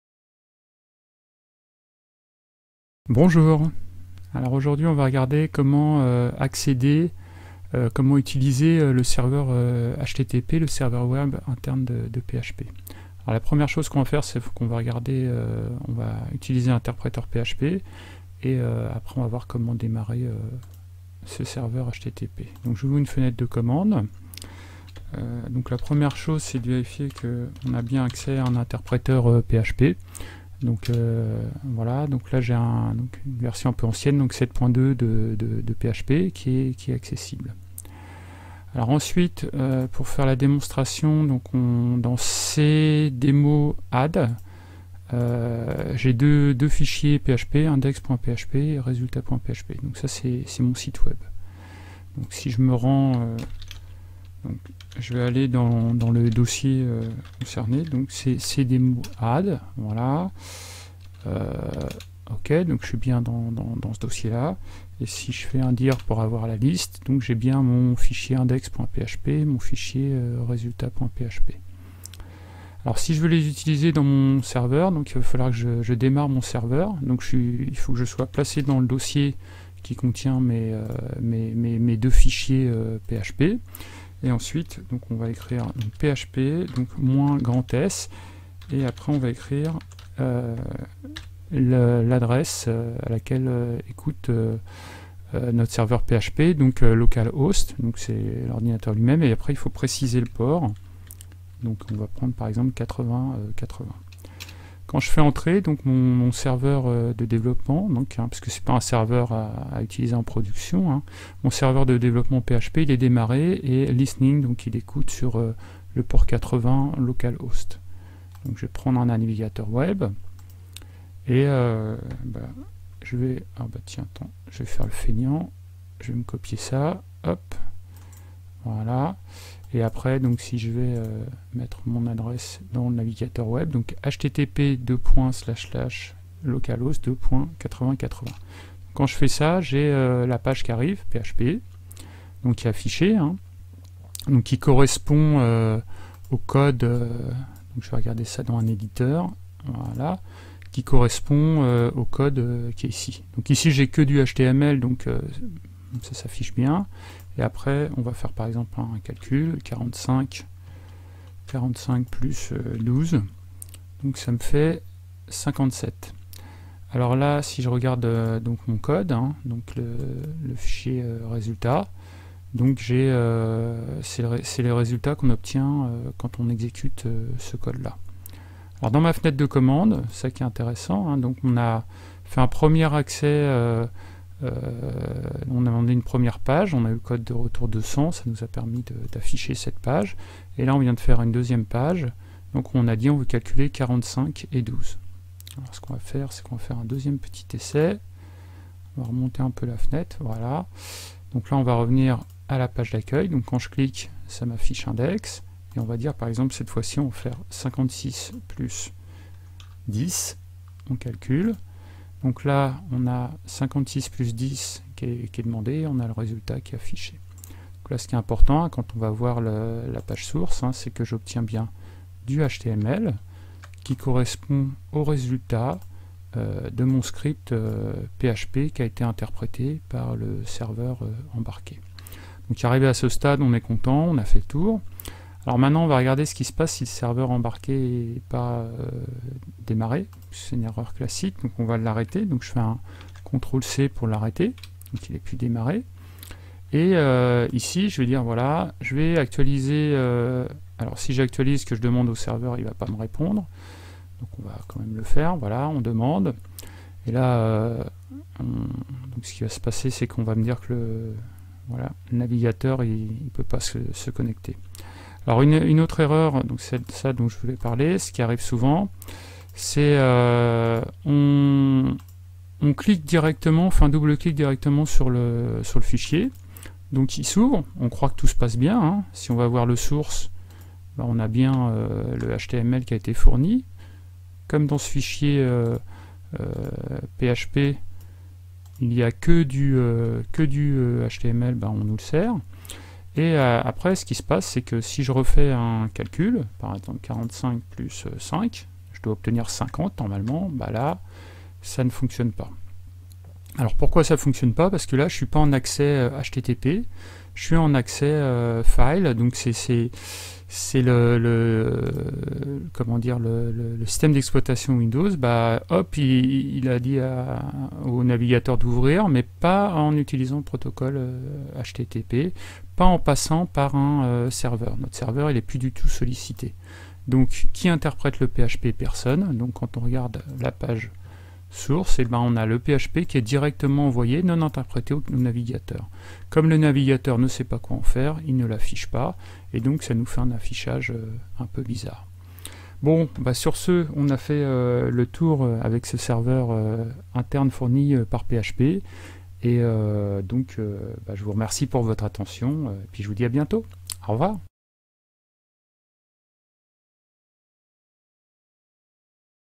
Serveur de développement de PHP
👉 Comment utiliser le serveur de développement de PHP ou serveur embarqué avec PHP.

📋 00:00 Objectif = Utiliser le serveur web de PHP
00:24 Premier démarrage de l'interpréteur php.exe
00:38 Présentation des fichiers fournis
00:54 Se déplacer dans le dossier contenant les fichiers fournis
01:12 Démarrer le serveur HTTP de PHP : php -S localhost:8080
02:00 Naviguer sur le site web
03:01 Voir les fichiers demandés au serveur web
03:56 Que se passe-t-il quand le serveur web est arrêté ?
04:34 Que se passe-t-il lorsqu'on double-clique sur un fichier PHP ?
05:11 Pourquoi accéder directement au fichier PHP ne fonctionne pas ?
05:50 Et ensuite ?